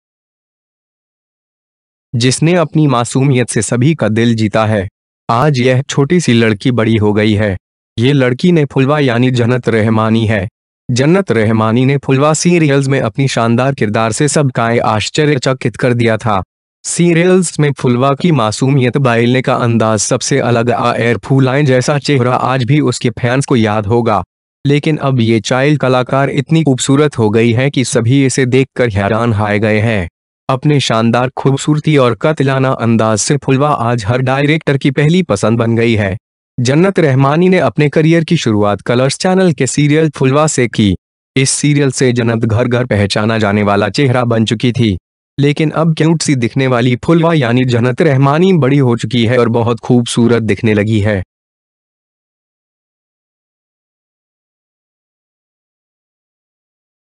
छोटी सी 'फुलवा' अब हो गयी है बड़ी, अब दिखती है ऐसी
आज यह छोटी सी लड़की बड़ी हो गई है। ये लड़की ने 'फुलवा' यानि जन्नत रहमानी है। जन्नत रहमानी ने फुलवा सीरियल्स में अपनी शानदार किरदार से सबकाे आश्चर्यचकित कर दिया था।

सीरियल्स में फुलवा की मासूमियत बाेलने का अंदाज सबसे अलग अाैर फूलाें जैसा चेहरा आज भी उसके फैंस को याद होगा।

लेकिन अब ये चाइल्ड कलाकार इतनी खूबसूरत हो गई है कि सभी इसे देखकर हैरान हाे गए है। अपने शानदार खुबसूरती और कातिलाना अंदाज से फुलवा आज हर डाइरेक्टर की पहली पसंद बन गई है।
जन्नत रहमानी ने अपने करियर की शुरूआत कलर्स चैनल के सीरियल 'फुलवा' से की। इस सीरियल से जन्नत घर-घर पहचाना जाने वाला चेहरा बन चुकी थी। लेकिन अब क्यूट सी दिखने वाली फुलवा यानि जन्नत रहमानी बड़ी हो चुकी है और बहुत खूबसूरत दिखने लगी है।